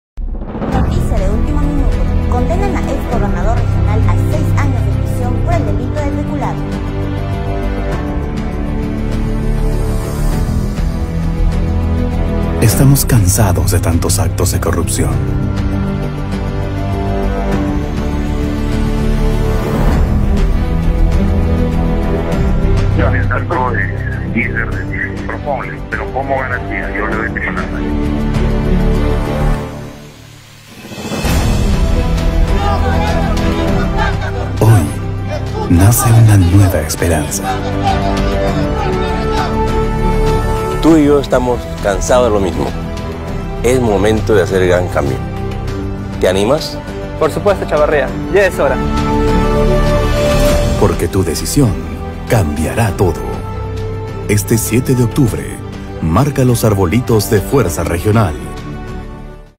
RICARDO CHAVARRIA: Tu Decisión Cambiará Todo
¡Vamos jóvenes de la Región! Es momento de hacerle frente a la corrupción, no permitamos que malos goberantes y funcionarios públicos sigan viviendo de nuestro esfuerzo y trabajo, es hora de tomar una desición y apostar por el cambio. Por un futuro mejor, este 07 de Octubre marca los 4 arbolitos de FuerzaRegional y vota por Chavarría para GobernadorRegional. ✍🌳🌳🌳🌳✔